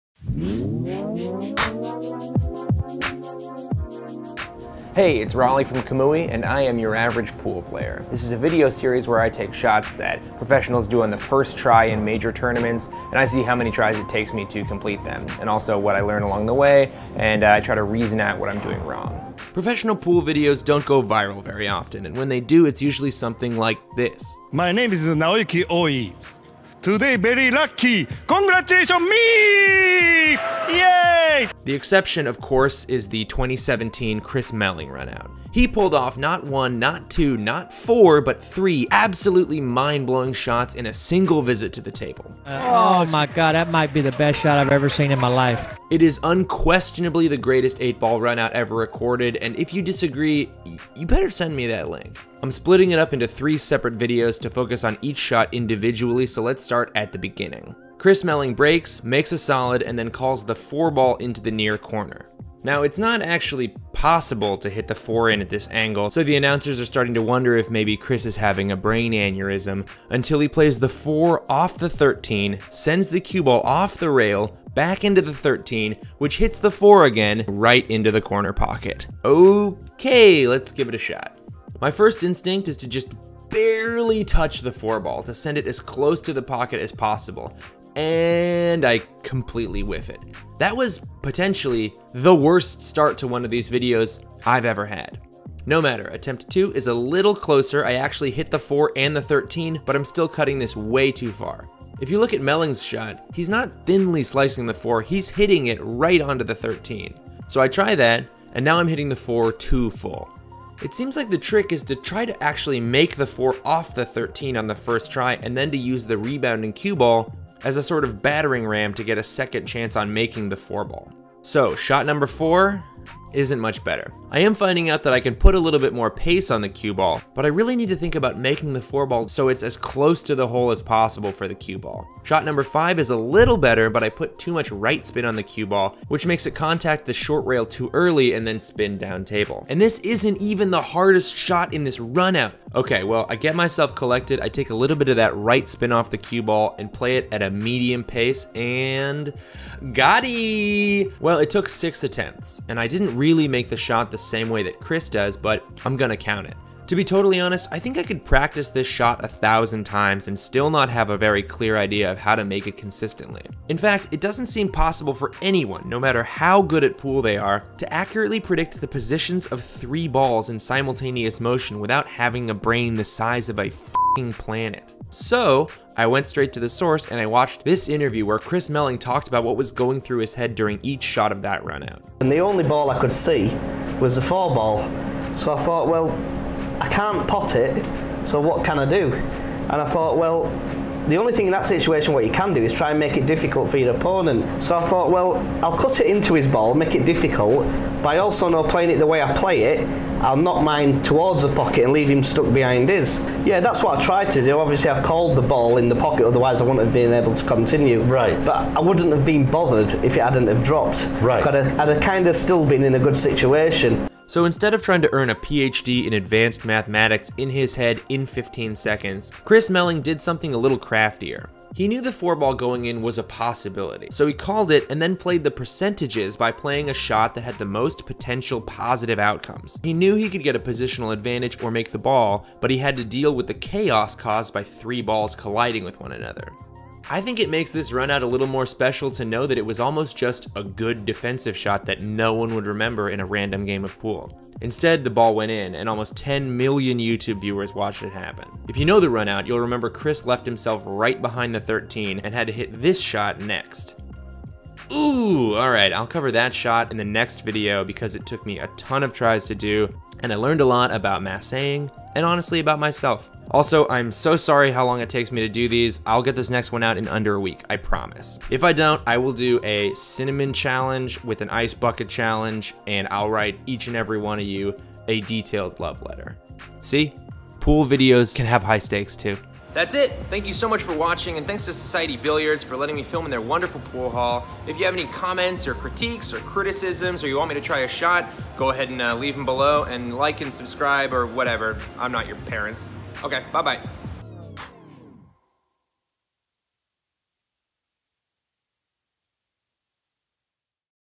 Trying The Chris Melling Runout [Part 1 of 3] | Your Average Pool Player
Even if you're not a diehard pool fan, there's a decent chance you heard about the Chris Melling runout in 2017. It was a series of unbelievable shots that culminated in one of the greatest 4-rail shots ever played in competition, and it lit the pool world up for months. In a very special 3-part Your Average Pool Player video, I try the bizarre time shot, kick shot, combo that Chris Melling drains to kick the whole thing off.